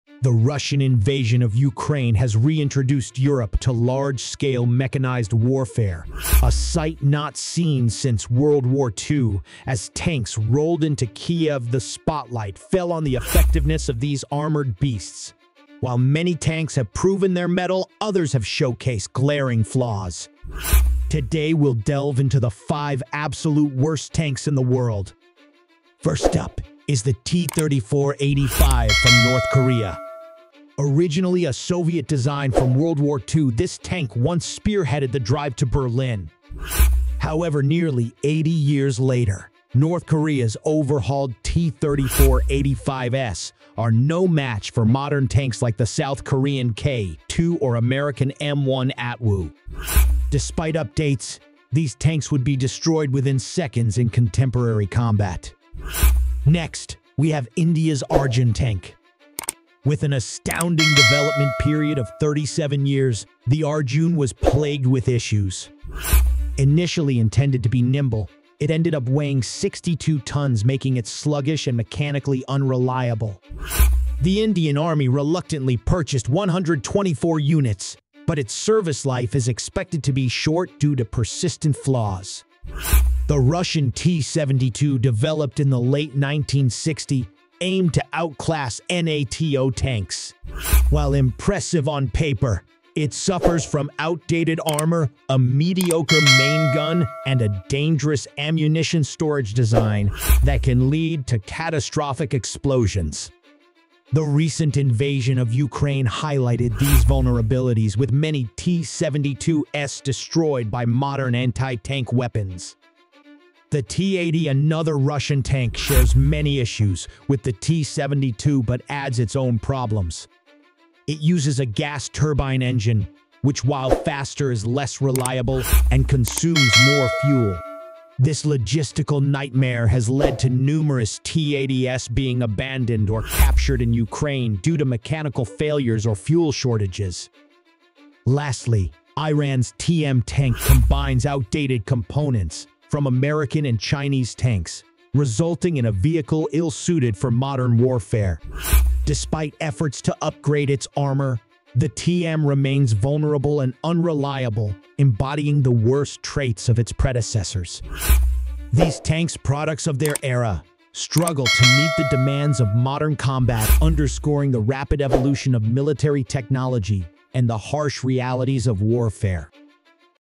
The 5 Absolute Worst Tanks in the World
The Russian invasion of Ukraine has spotlighted the effectiveness and flaws of various tanks. Among the worst are North Korea’s outdated T-34/85, India’s sluggish and unreliable Arjun, Russia’s vulnerable T-72 and T-80, and Iran’s Tiam, which combines outdated components from older tanks. These tanks struggle to meet modern combat demands, revealing their significant shortcomings in today's warfare.

Follow us on other social media platforms